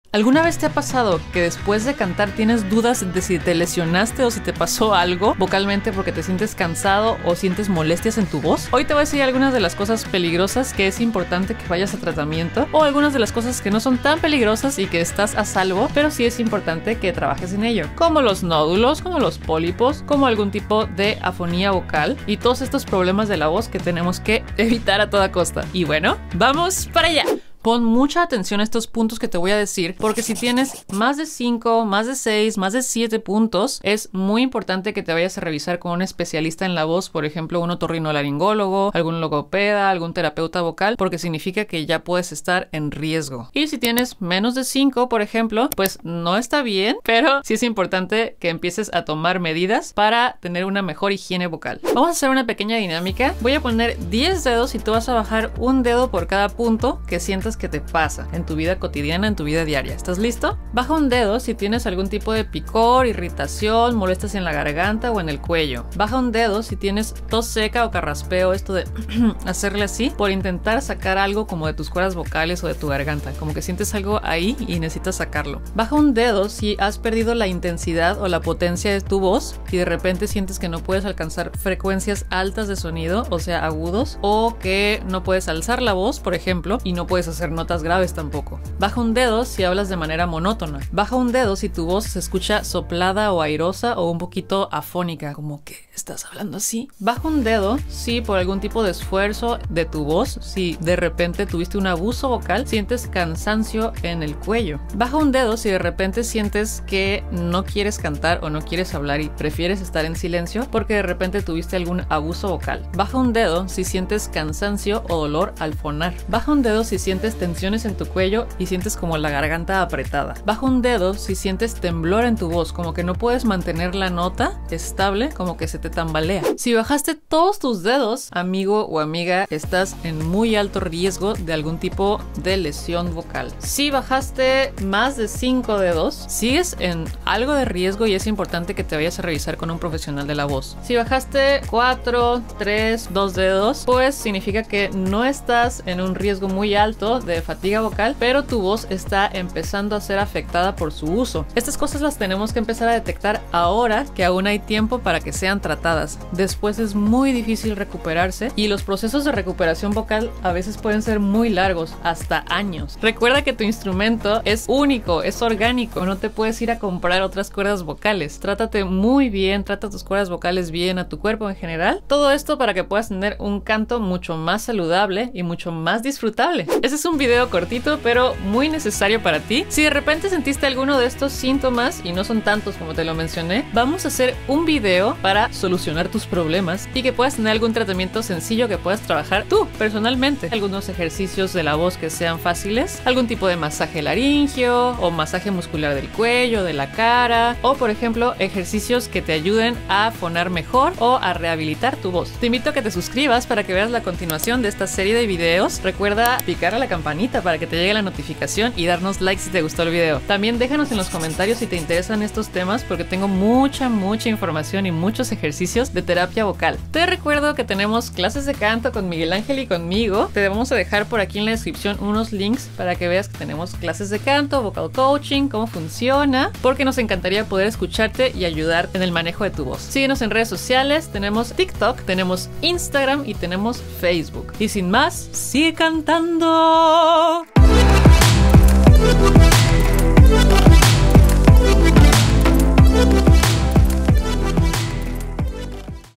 Sintomas de Fatiga Vocal - Voz Lastimada - Esfuerzo Vocal - Como Identificar Una Voz Cansada "Canto"
Hot hablaremos y vamos a identificar los sintomas de una fatiga vocal, así que si haz hecho un esfuerzo vocal y no estas seguro de tener una voz lastimada o cansada, te invito a ver este video.